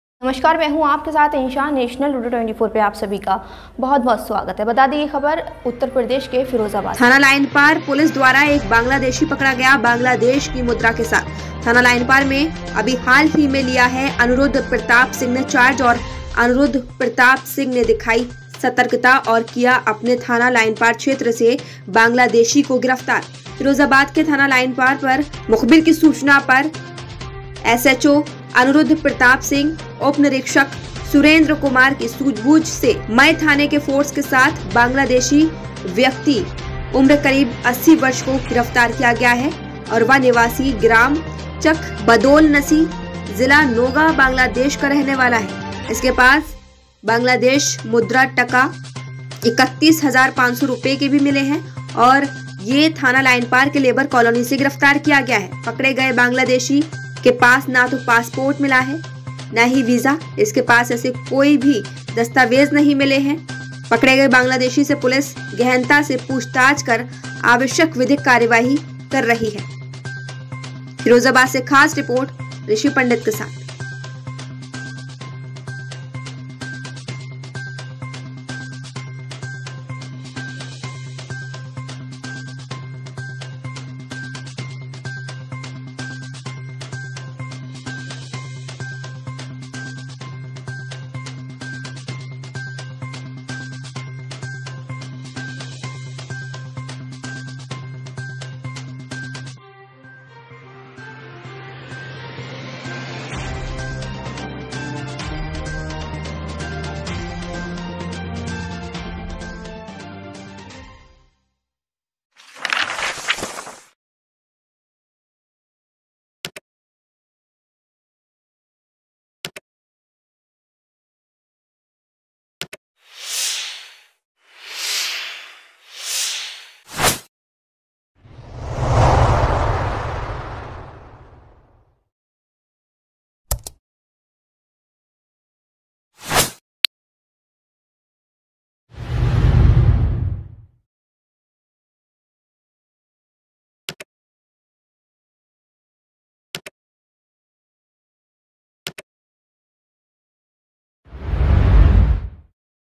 bangladeshi_थाना लाइनपार पुलिस ने लेबर कॉलोनी में बांग्लादेशी पकड़ा, बांग्लादेश की मुद्रा के साथ
थाना लाइनपार पुलिस ने लेबर कॉलोनी में बांग्लादेशी पकड़ा, बांग्लादेश की मुद्रा के साथ।